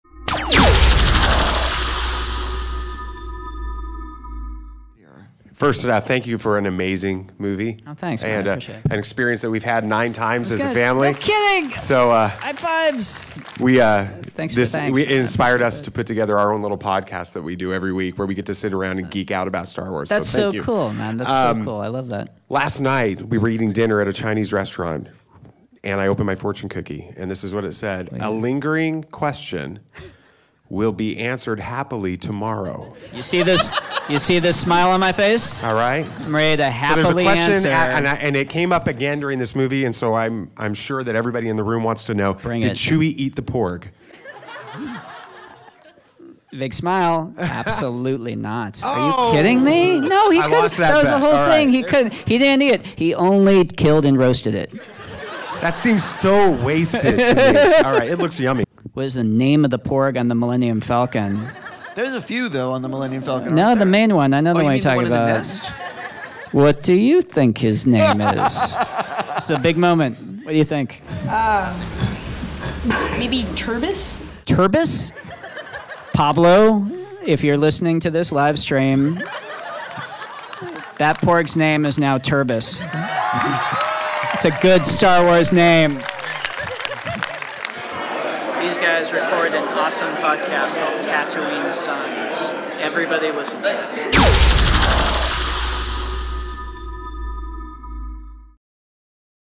Rian Johnson lets a KID name a Star Wars character!!!
We've had a pretty great couple of weeks! We got invited by Collider to Los Angeles to visit IMAX HQ for a special screening of Star Wars: The Last Jedi, followed by an extended conversation with the movie's writer/director, Rian Johnson! But WAIT! There's more!

Steven Weintraub (Collider) opened the floor up for questions for the audience! WHAT?!?!

First question: The Bow Tie Jedi Guy (David Jesse) kicked it off with a question inspired by his fortune cookie received the night before. Let's just say, it was about a certain Wookie's culinary choices!

But then, BB-N8 (Nathan Jesse) turned EVERYTHING upside down when he asked Rian, "What is the name of the porg on the Millennium Falcon?" Rian didn't have an answer, but BB-N8 did!

Finally, we talked with Rian in the lobby, and received a very special THUMBS UP for our weekly Star Wars podcast!

A special thanks to Collider for giving us the special moment! And a VERY SPECIAL thanks to Rian Johnson for making it more special than we could ever have imagined!